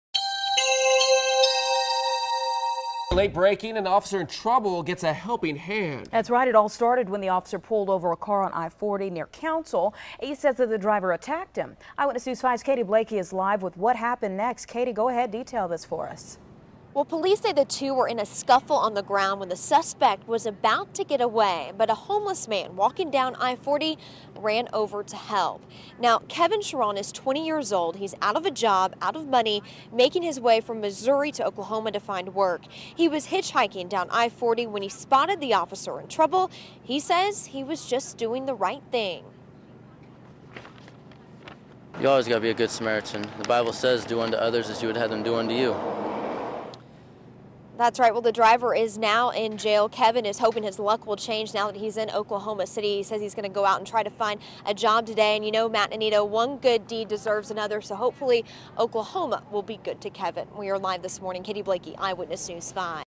Homeless Man Helps Officer Make Arrest
Police say a homeless man came to help when he saw an officer involved in a scuffle after a traffic stop on Wednesday morning.